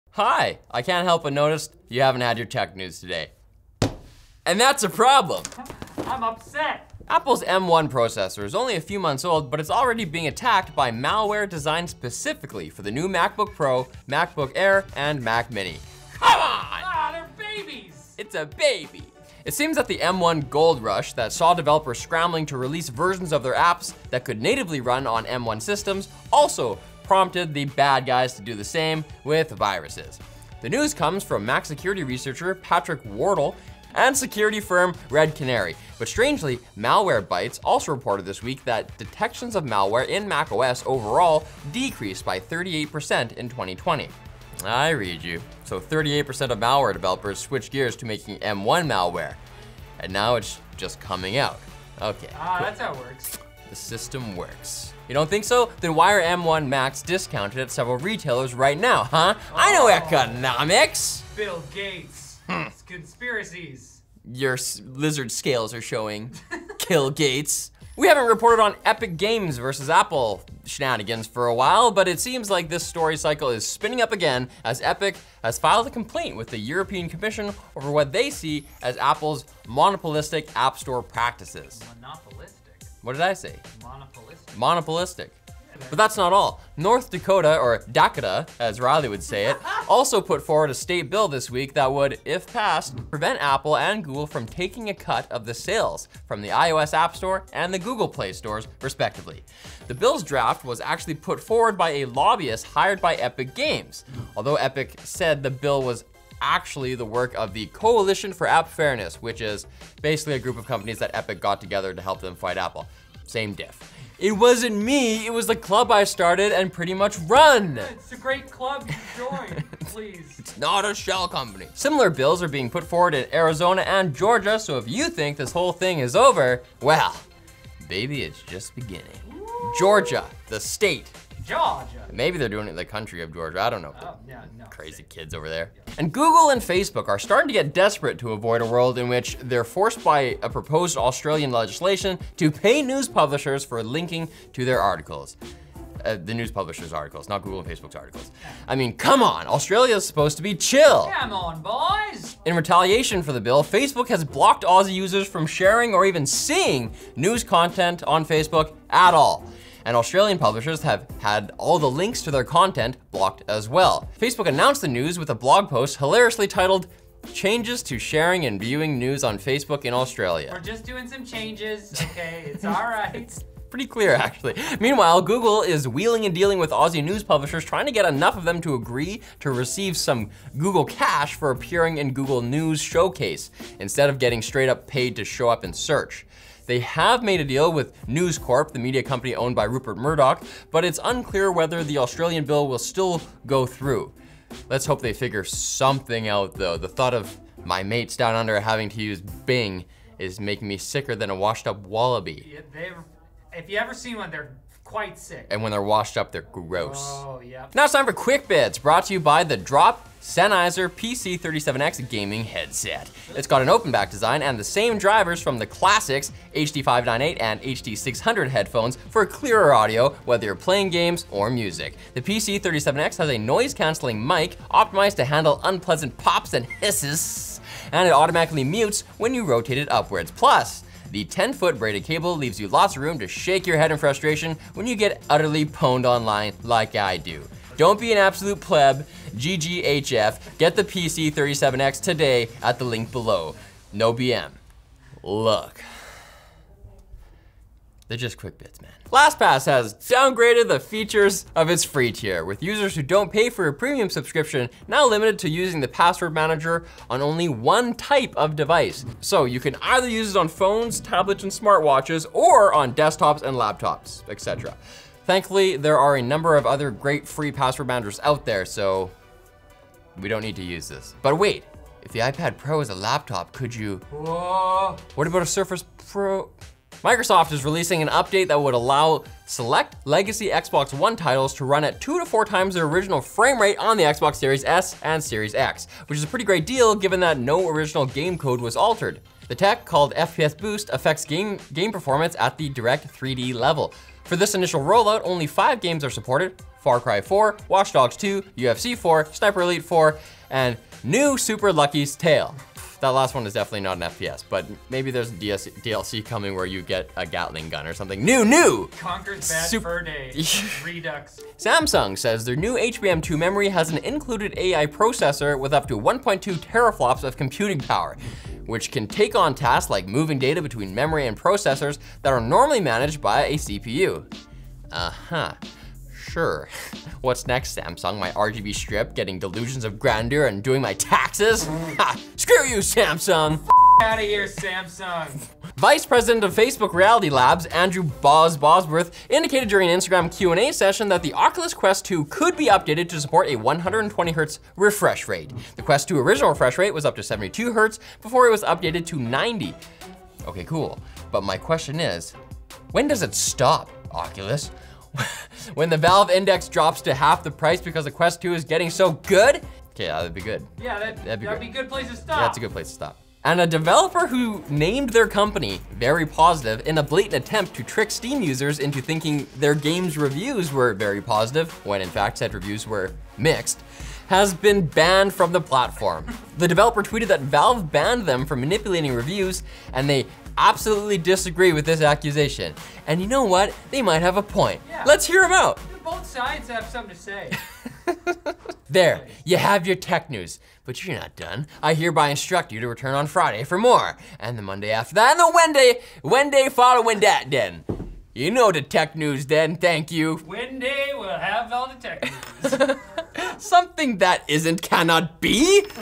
But… Macs don’t get VIRUSES…
Timestamps:
0:00 everybody needs tech news
0:07 M1 Mac viruses
1:07 Epic fights Apple in EU
2:15 Facebook blocks news in Australia
3:27 Sennheiser PC37X gaming headset
4:04 QUICK BITS
4:09 Lastpass downgrades free tier
4:36 Xbox FPS Boost
5:11 Samsung HBM2 Smart memory
5:35 Oculus Quest 2 120Hz ?
6:03 "Very Positive" Steam dev banned

NEWS SOURCES

WELL WHY DID I BUY A MAC THEN
But M1 Macs don’t get viruses
and the M1 Mac Mini is discounted, COINCIDENCE??

EPIC DETERMINATION
Epic Games is still tryin

BLOCK THE HATERS
Facebook blocks users from sharing news links in Australia
“Changes to Sharing and Viewing News on Facebook in Australia”

QUICK BITS

LAST STRAW
Lastpass restricts free plan users to 1 type of device

NOSTALGIA BOOST
Xbox FPS boost

SMART MEMORY
Samsung HBM2 has 1.2 TFLOPS of processing power

THE QUEST CONTINUES?
Oculus Quest 2 could get another refresh rate update to 120Hz

I’M POSITIVE THAT’S CHEATING
“Very Positive” Steam developer banned